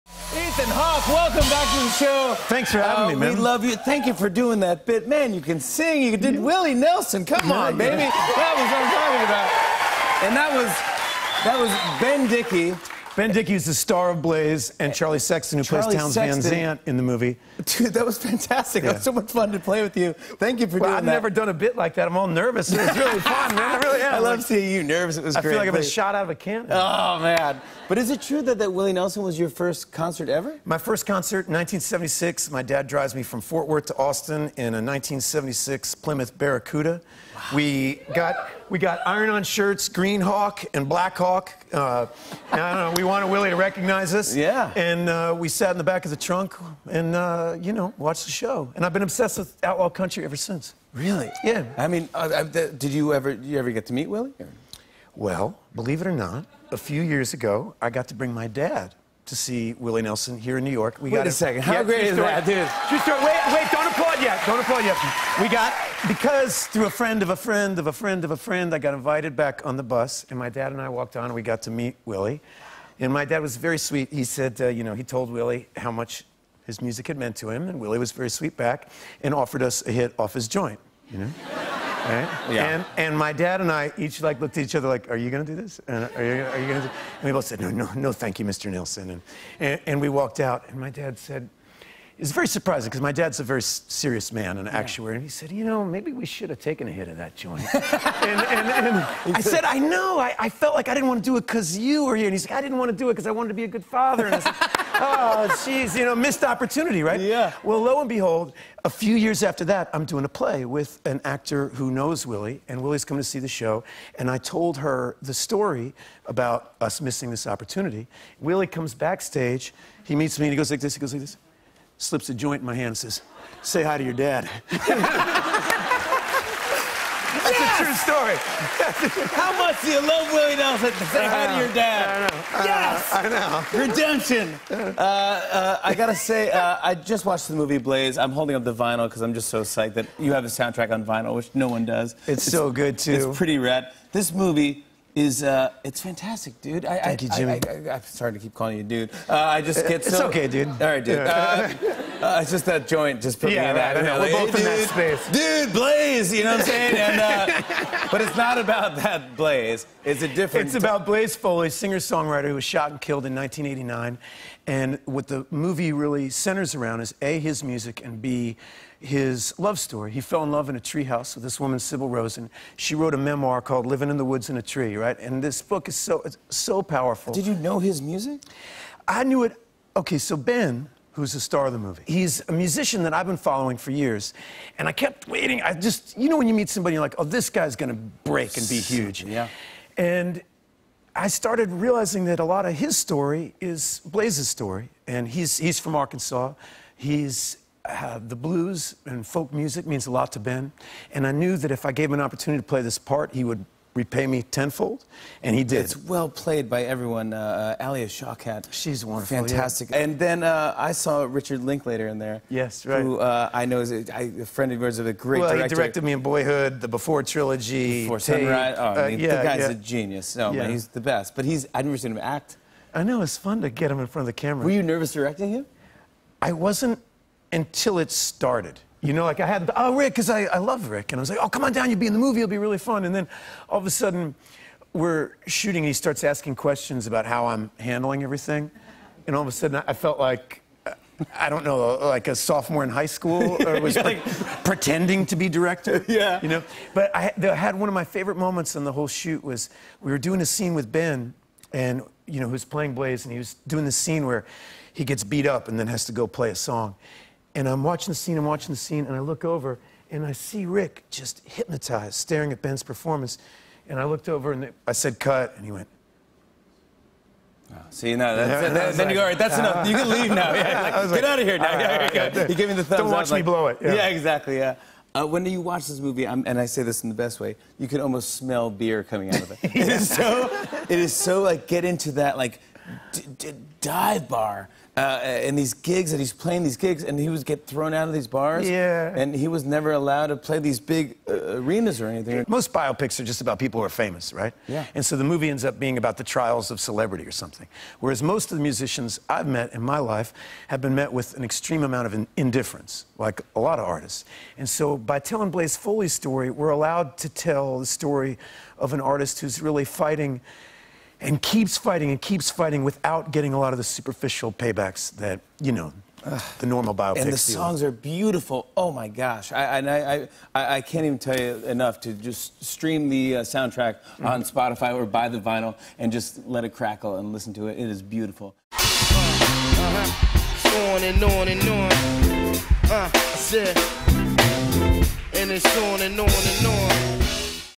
Ethan Hawke Got a Second Chance to Smoke a Willie Nelson Joint
Ethan Hawke talks to Jimmy about how a missed opportunity with his dad led to a drug exchange with Willie Nelson, and he discusses how singer-songwriter Ben Dickey was the perfect musician for his biopic Blaze.